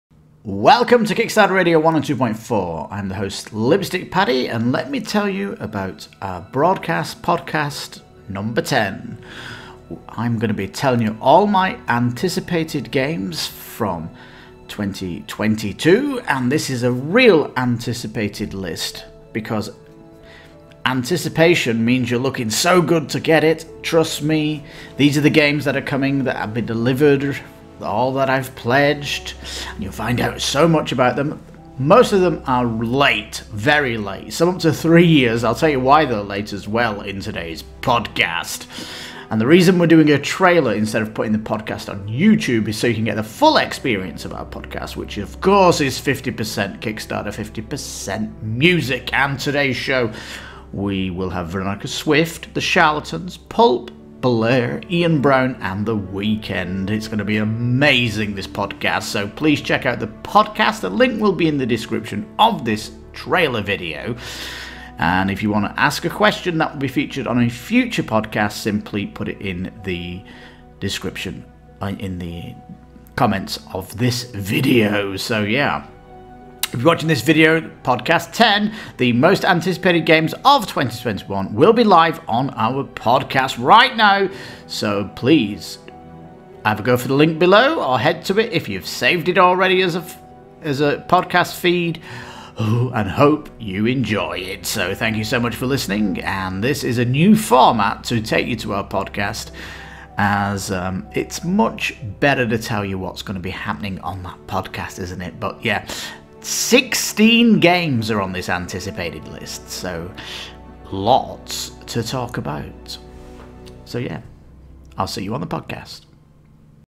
🎼Broadcast 10🎲 "Most Anticipated" Board Games 2022 TRAILER
The truly "Most Anticipated" board games are the ones you've pre-ordered or pledged for.  One you cannot wait to unbox, unwrap, punch out, bag up and read the rulebook in true anticipation for your first play through.  Magical, exciting and memorable to say the least.

Our host looks at all the games he pledged for coming in 2022 and share's his excitement for them.

Also music from: Veronica Swift, The Charlatans, Pulp, Blur, Ian Brown, and The Weeknd.

Got Bored of Retail?

🎙Kickstarter Radio 102.4
Produced and Presented by Lipstickpaddy

SOCIAL